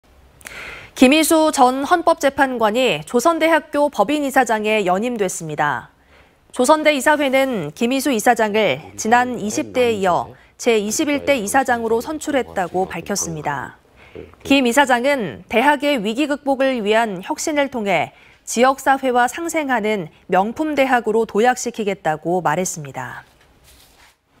김이수 전 헌법재판관, 조선대 이사장 연임
김이수 전 헌법재판관이 조선대학교 법인 이사장에 연임됐습니다.

조선대 이사회는 김이수 이사장을
지난 20대에 이어 제21대 이사장으로 선출했다고 밝혔습니다.

김 이사장은 "대학의 위기 극복을 위한 혁신을 통해 지역사회와 상생하는 명품대학으로 도약시키겠다"고 말했습니다.